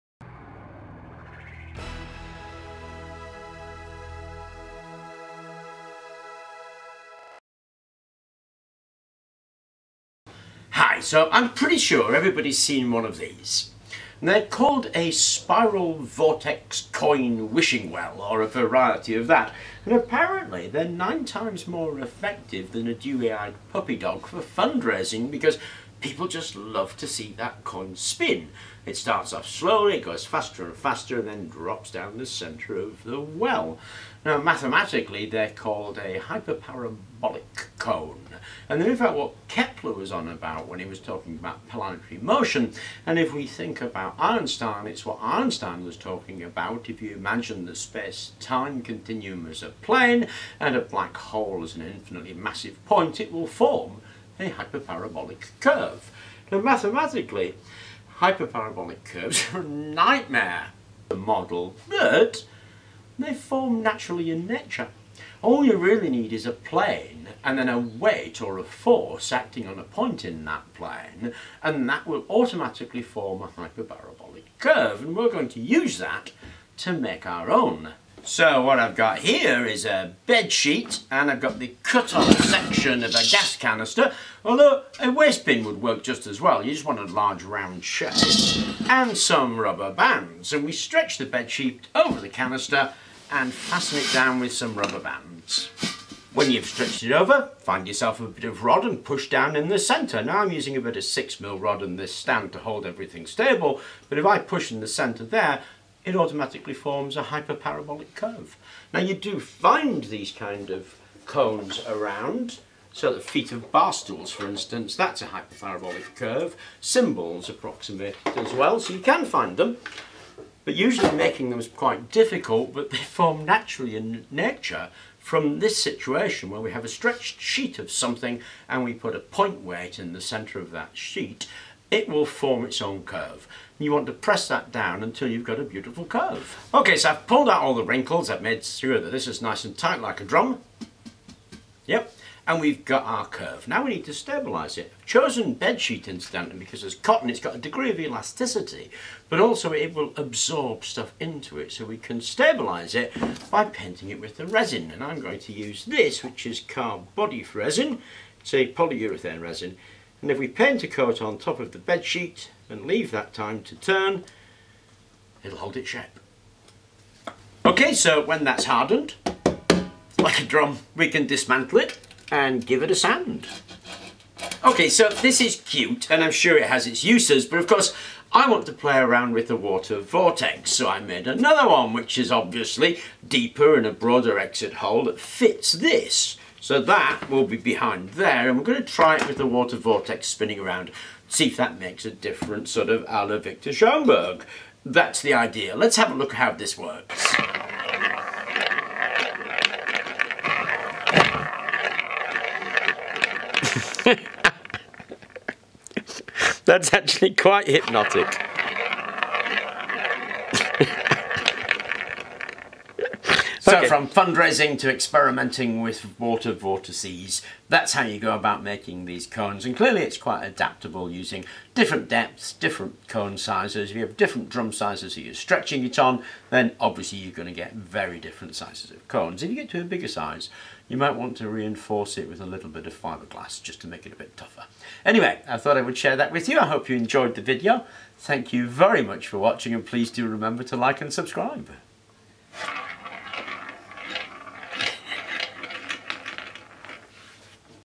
1490 Vortex Power - How To Easily Make A Hyperbolic Funnel
you actually see these all the time in spiral wishing wells and coin collection boxes a quick test of this with a wind turbine is